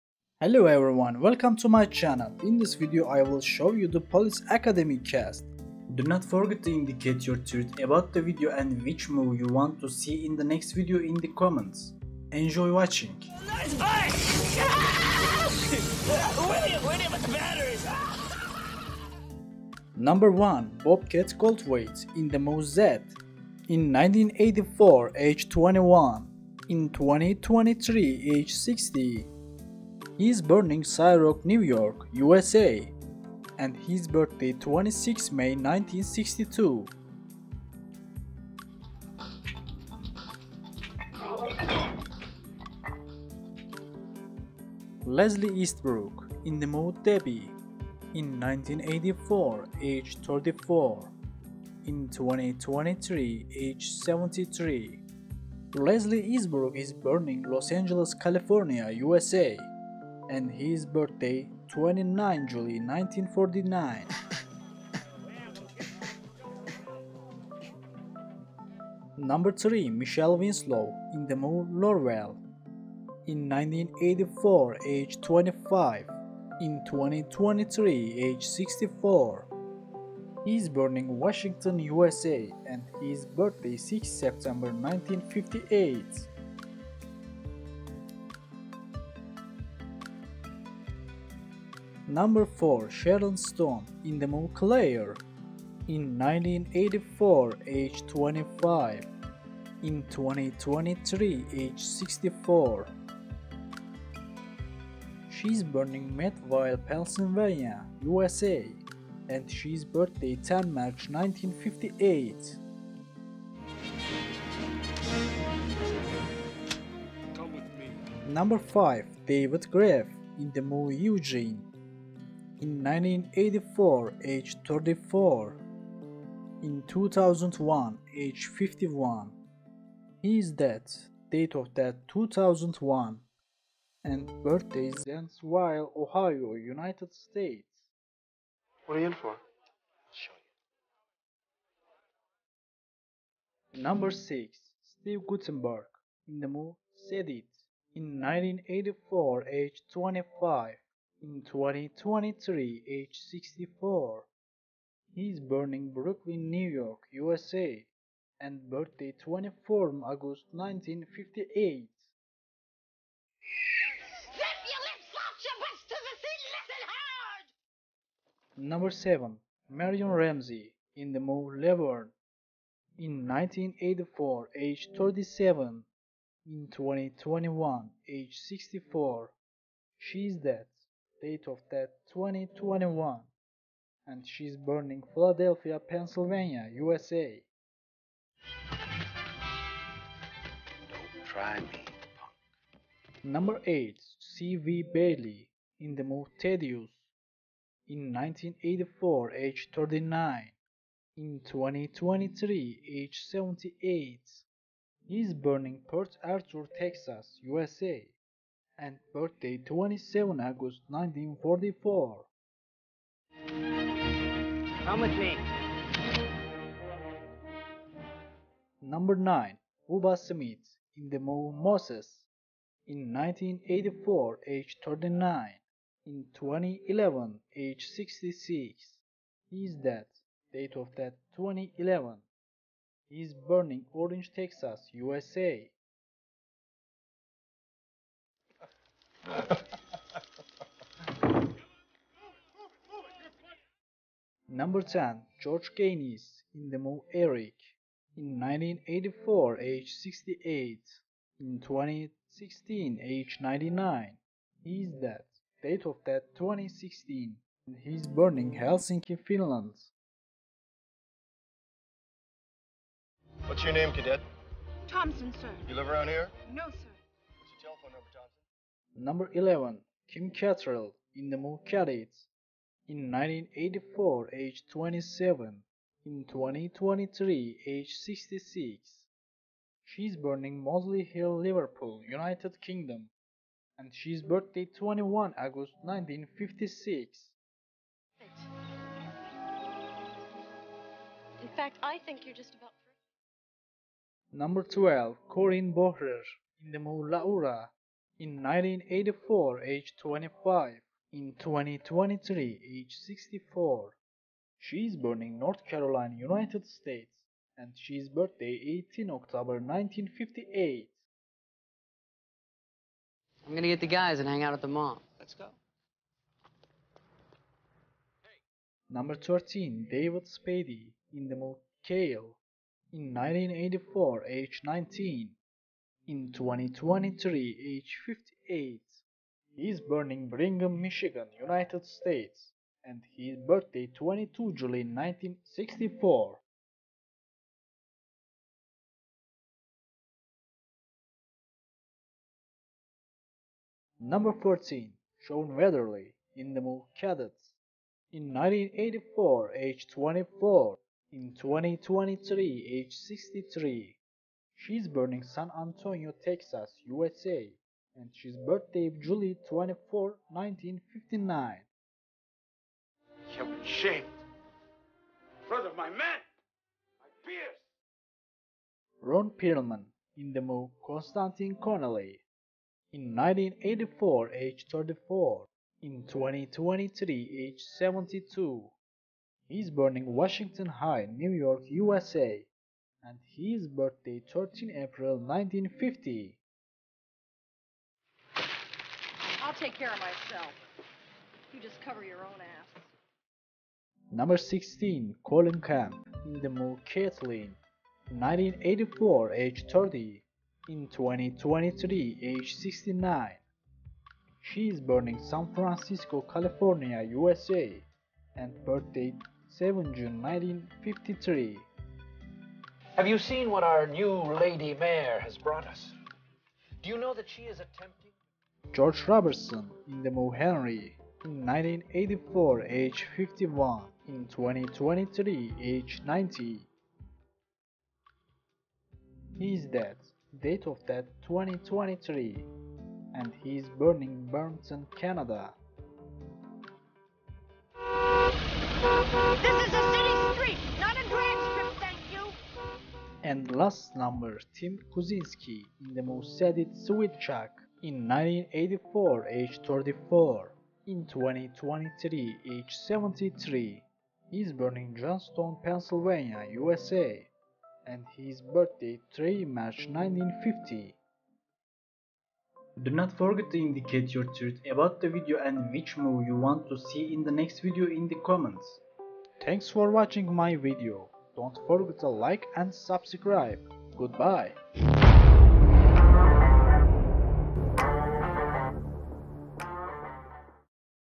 Police Academy ★ Cast Then And Now (1984 - 2023) Real Name And Age
In this video, we take a look at how the cast of Police Academy has changed over the years. We look at the real age and name of each actor and compare them to their role in the 1984 movie.

Then and now, it's time to check out the cast of Police Academy! In this video, we compare the actors playing the roles of zed, debbie callahan, cadet thompson, and claire mattson in 1984 to see how their looks and ages have changed. Don't miss out on this fascinating look at how the cast of Police Academy has evolved over the years!

Police Academy is a series of American comedy films, the first six of which were made in the 1980s and the seventh in 1994. The series opened with Police Academy, which started with the premise that a new mayor had announced a policy requiring the police department to accept all willing recruits.

👇actors 👇

★ Bobcat Goldthwait
★ Leslie Easterbrook
★ Michael Winslow
★ Sharon Stone
★ Kim Cattrall
★ Corinne Bohrer